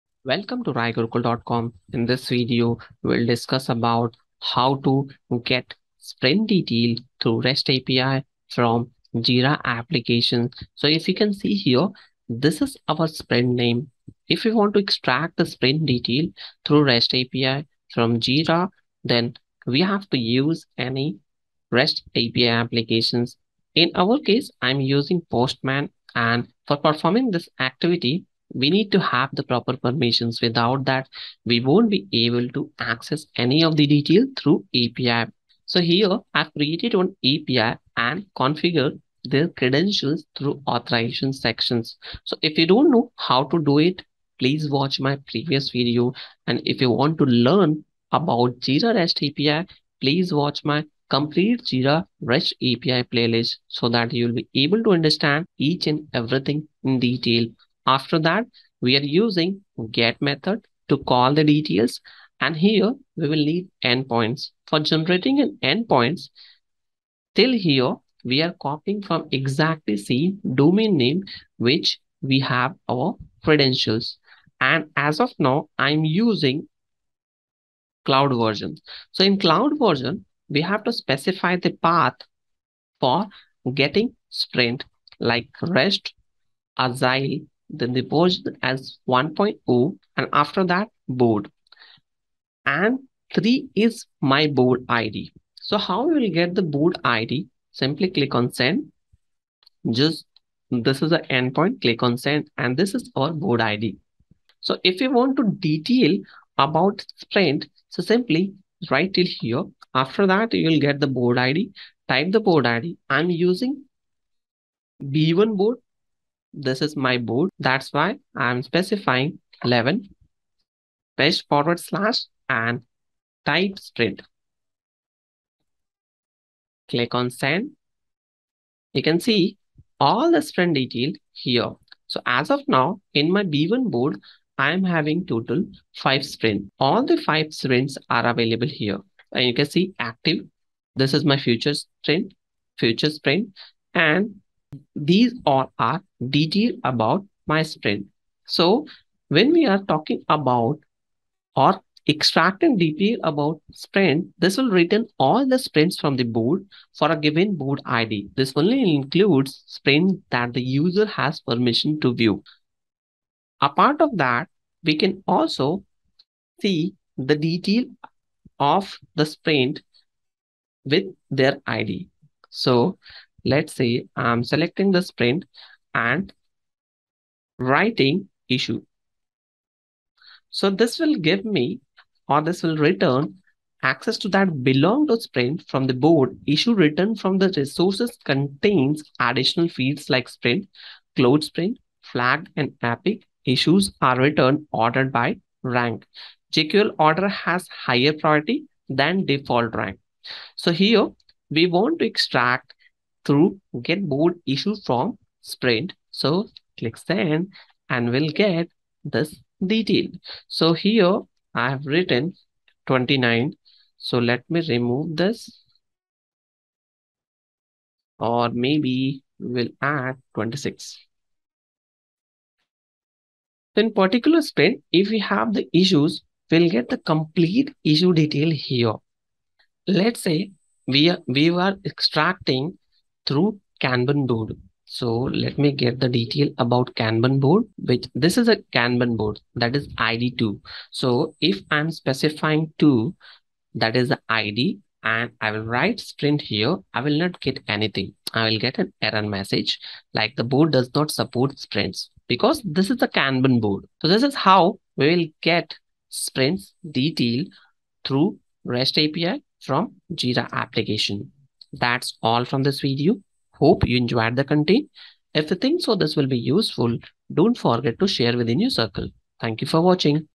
How to Get Jira Sprint Detail through REST API
In this video, I'll show you how to use the Jira REST API to get the Sprint detail you're looking for. With this technique, you'll be able to interact with Jira through a API interface rather than having to use the Jira web interface.

If you're looking to manage your Jira projects in a more efficient way, then this technique is for you! By using the Jira REST API, you'll be able to access information about your projects in a more organized way. Don't wait any longer; watch this video and learn how to get the feature you're looking for through the Jira REST API!